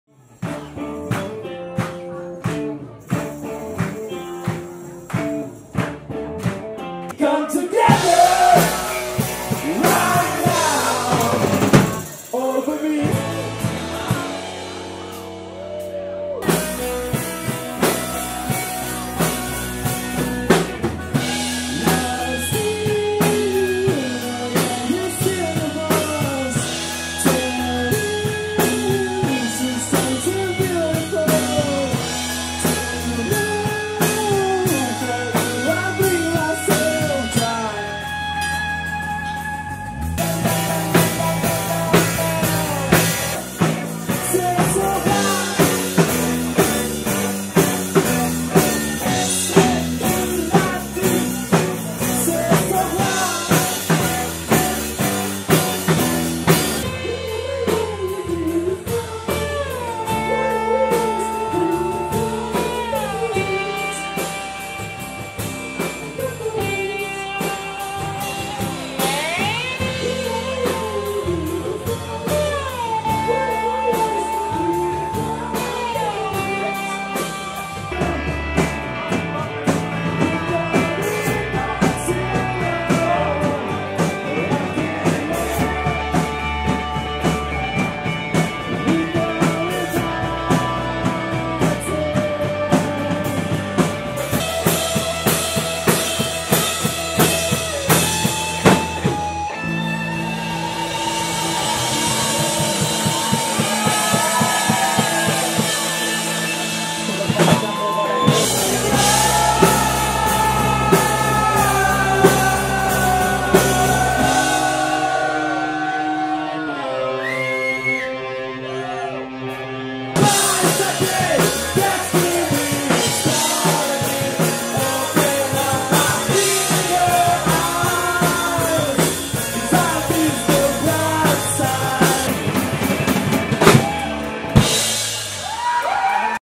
UK Sessions - Vikings Brew Pub - 19/12/2020
Trechos da apresentação no Vikings Brew Pub, em Anápolis, dia 19 de dezembro de 2020.